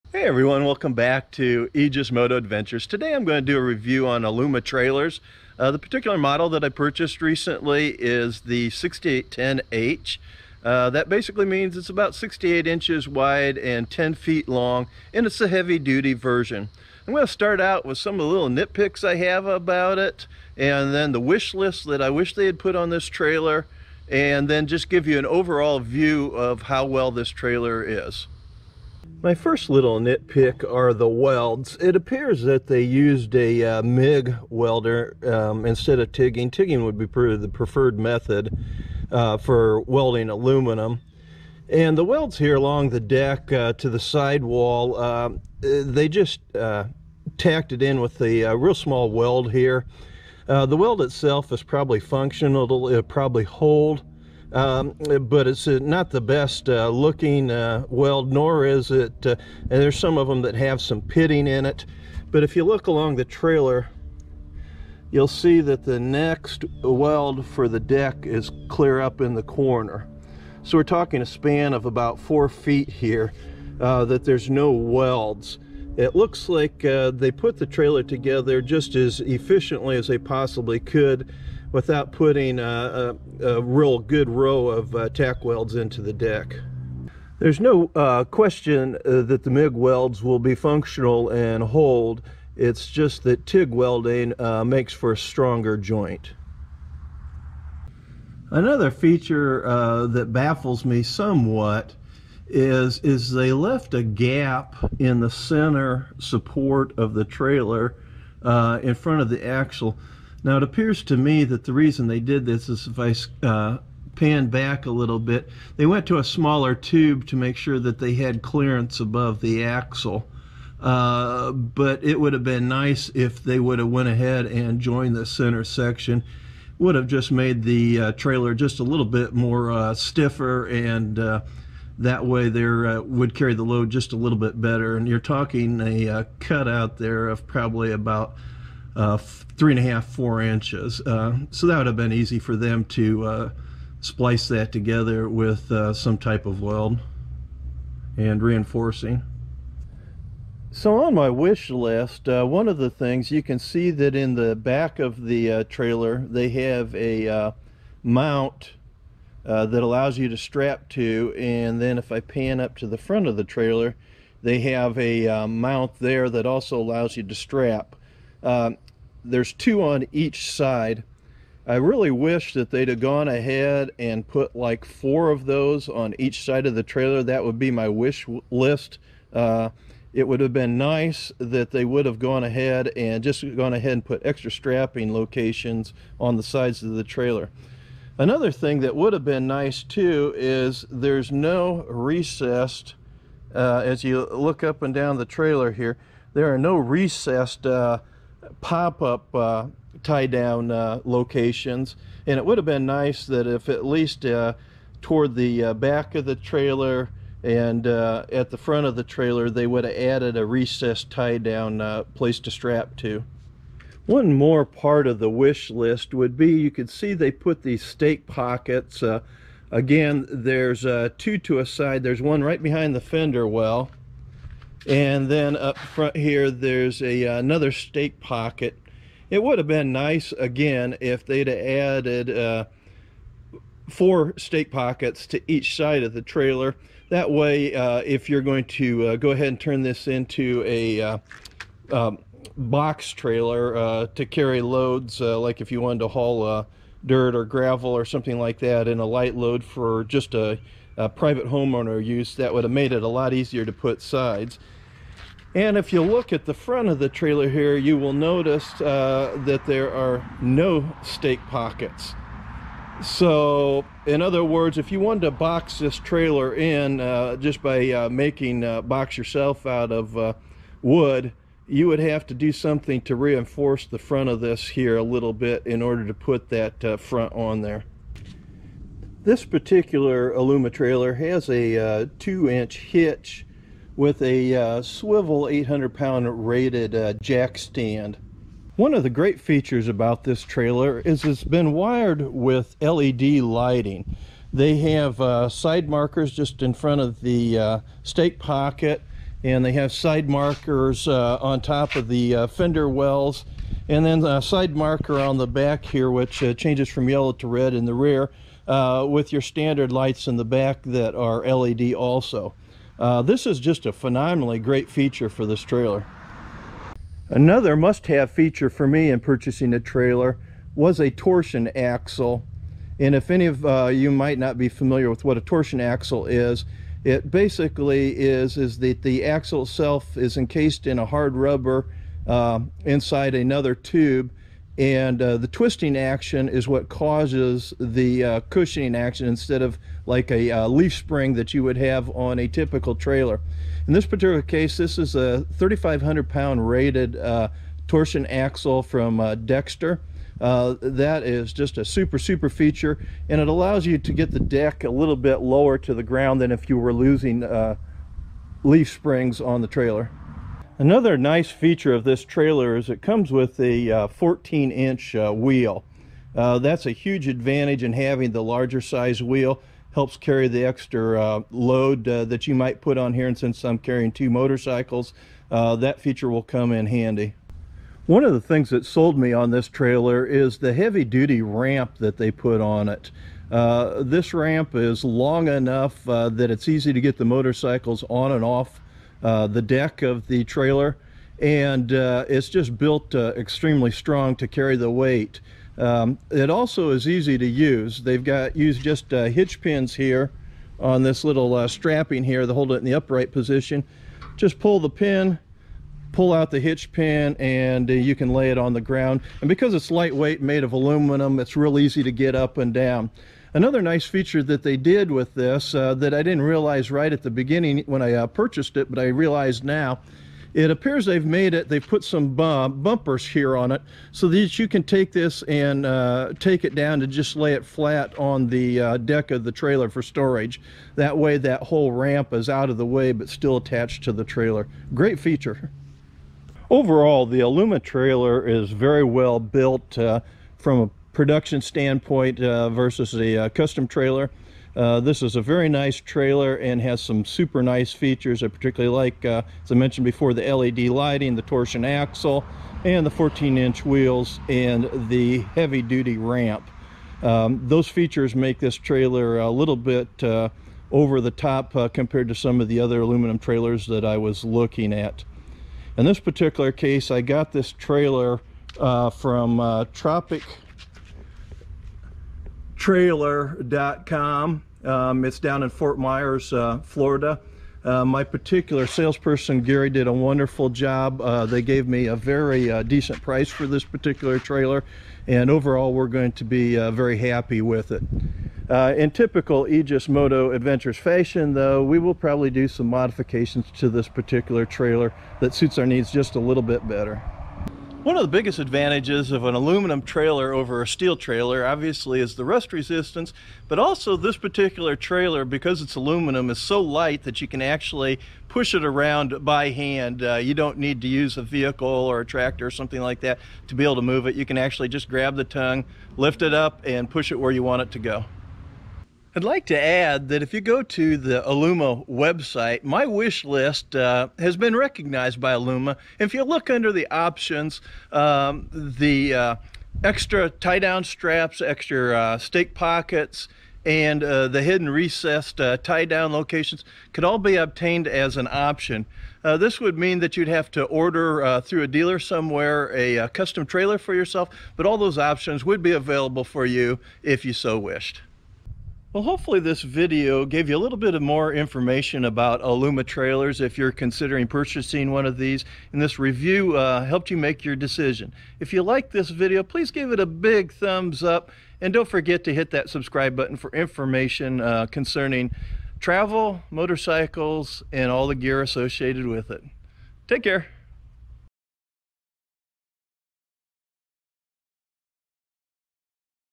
Aluma Trailer Review
Mike from Aegis Moto Adventures reviews Aluma Trailers- Motorcycle Adventure Travel